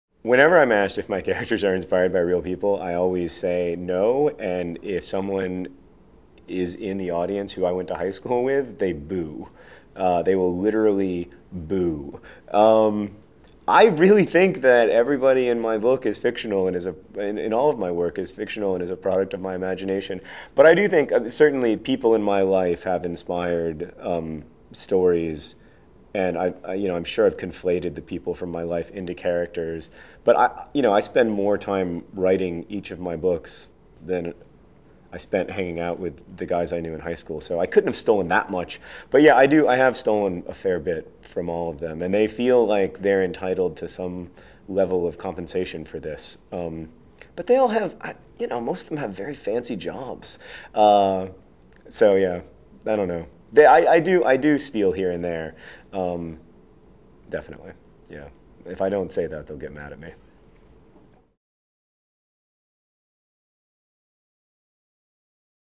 Author John Green: Don't I know you from somewhere?
John Green doesn't think his characters are based on real people, but his high school friends disagree.

For more information about teaching reading and writing to adolescents